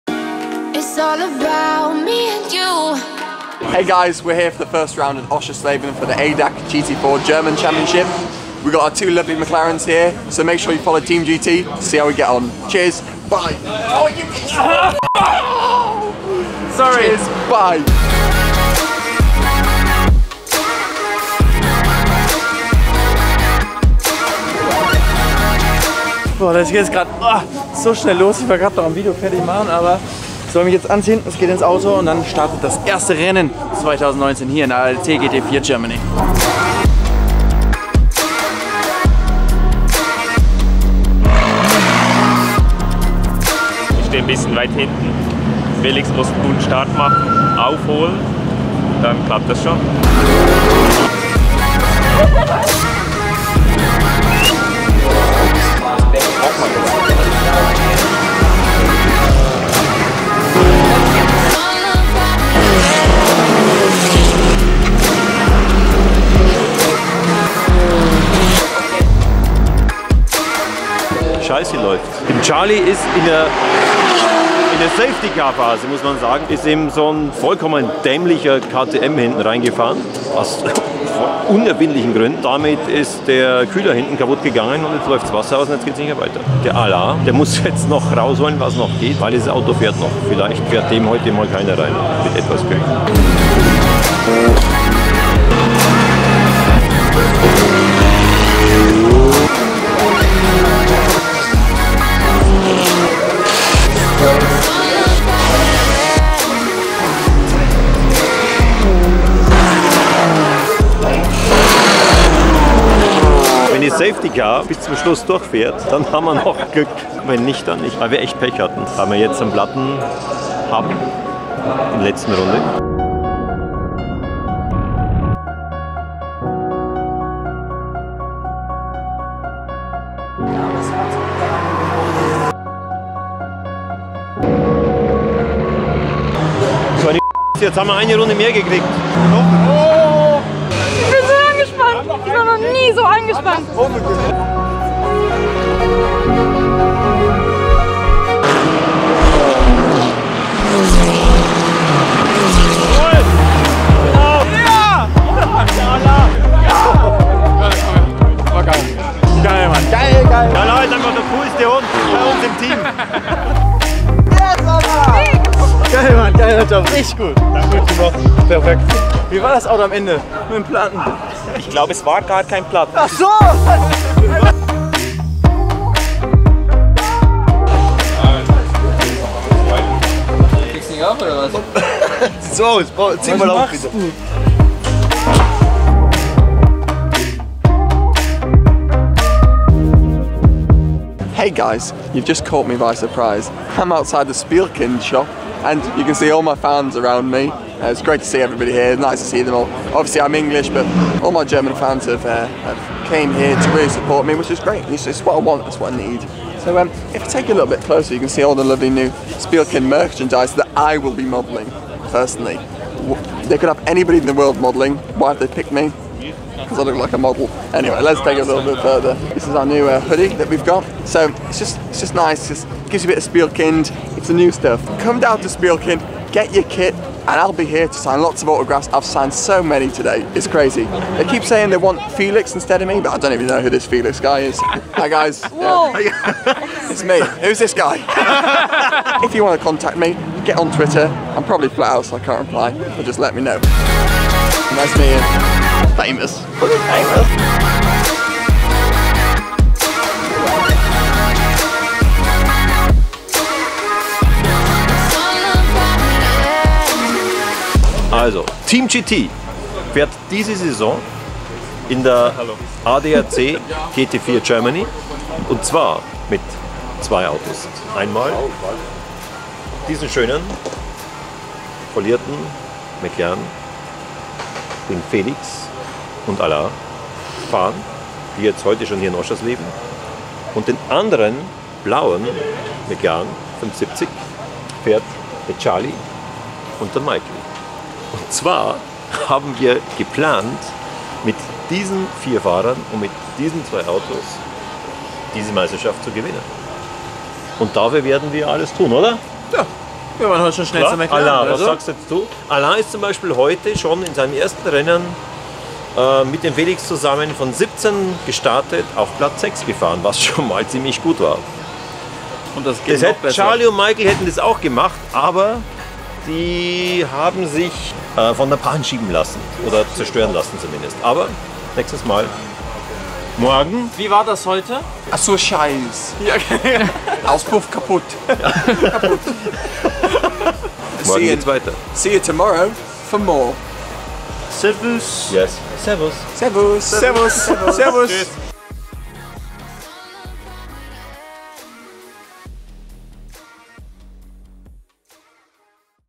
P6 Drama für Alain und Felix | McLaren Team GT
Tag 1 von Team GT's erste, Rennwochenende in Oschersleben mit Felix von der Laden, Charlie Fagg, Michael Benyahia und Alain Valente.